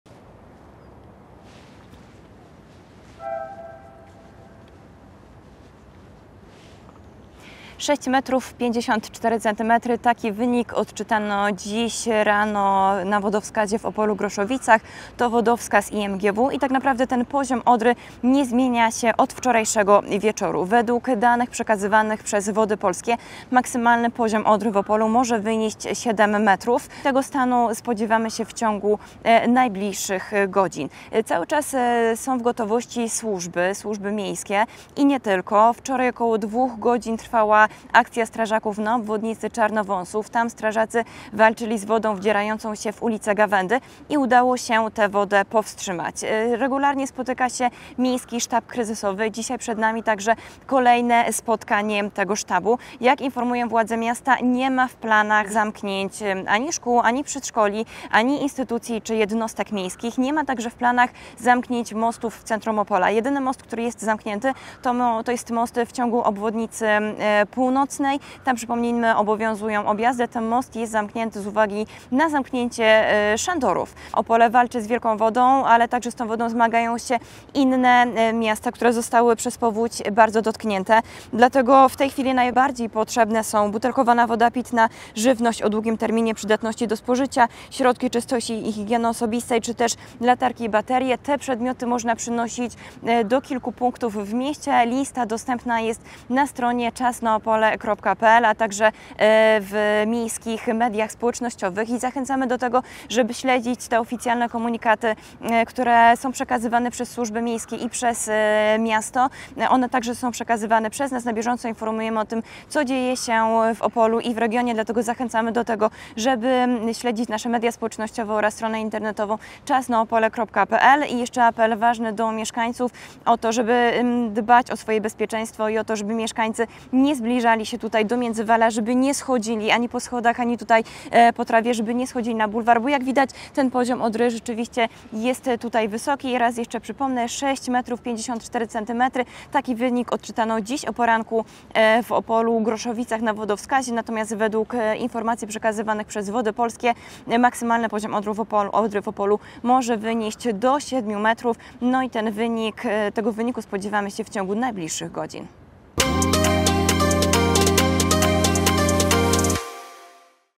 Sytuacja hydrologiczna w Opolu - stan na 8.30
W ciągu kolejnych godzin zgodnie z danymi Wód Polskich w szczytowym momencie poziom Odry nie przekroczy 7m. Tego stanu spodziewamy się w ciągu kolejnych godzin. Sprawdzamy sytuację hydrologiczną w mieście.
📰Szukasz więcej informacji na temat?